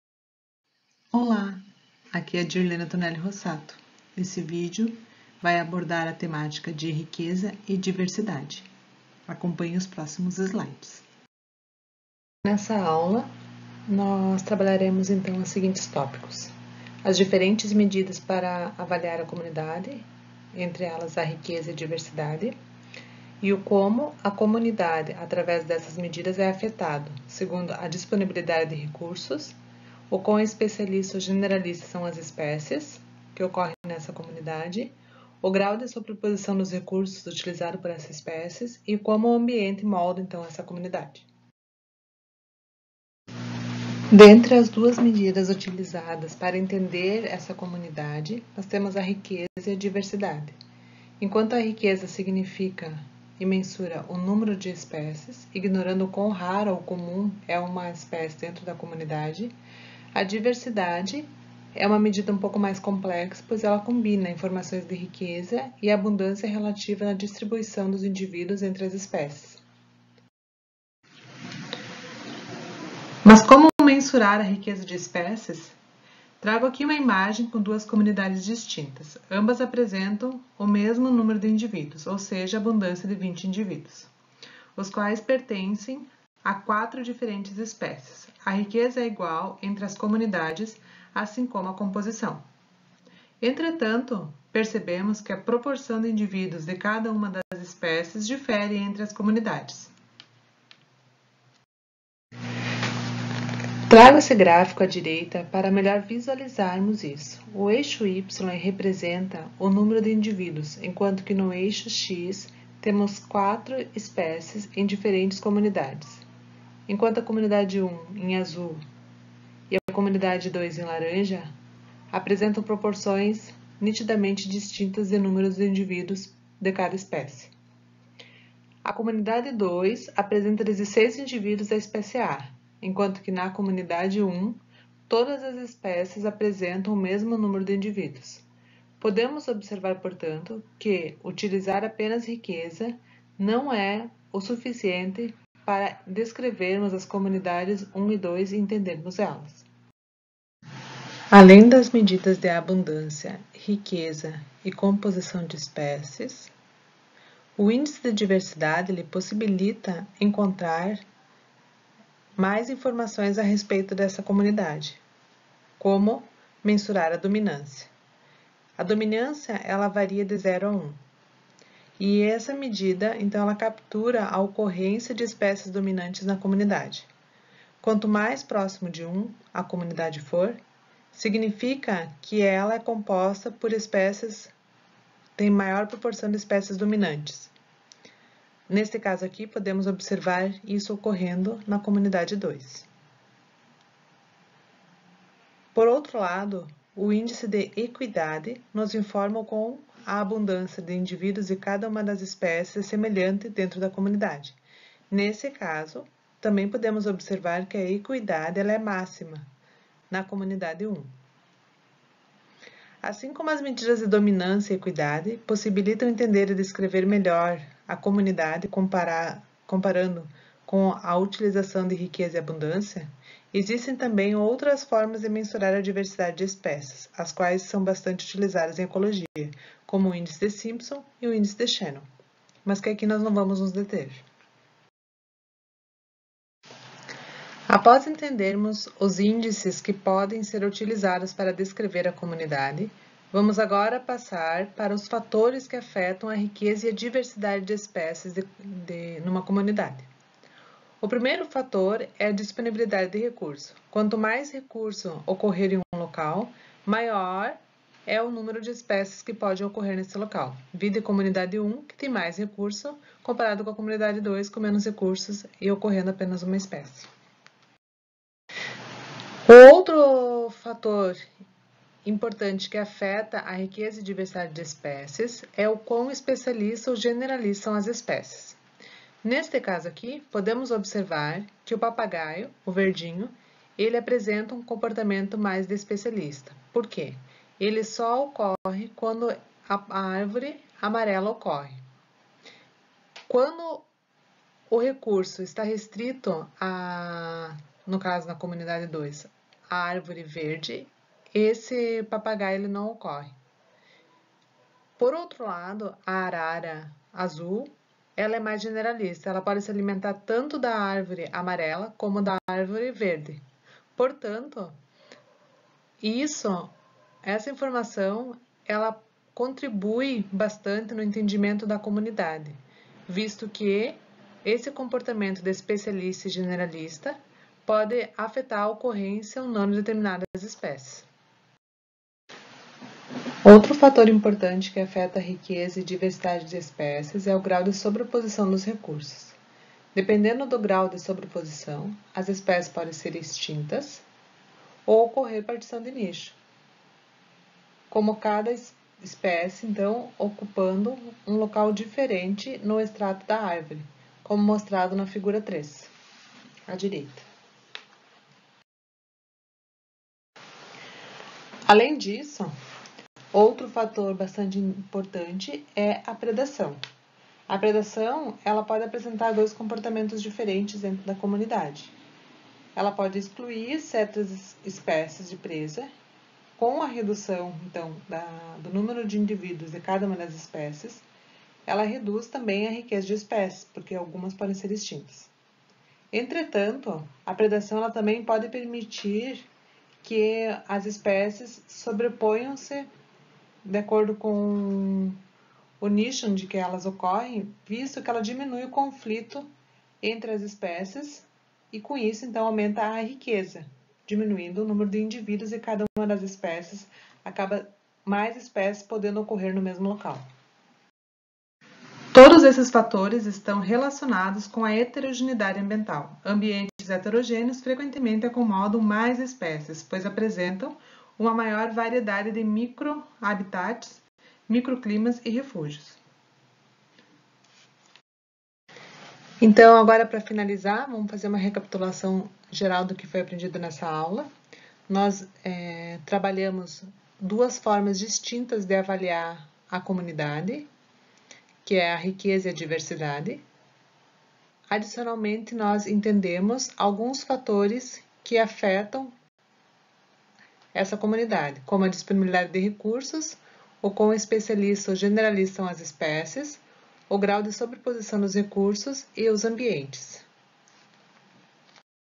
Diversidade e riqueza de espécies
A riqueza e a diversidade de espécies é uma medida bastante útil para avaliar diversos impactos sobre a biodiversidade. Mas como acessar ela?

Deixe seus comentários e dúvidas que terei o maior prazer em responder.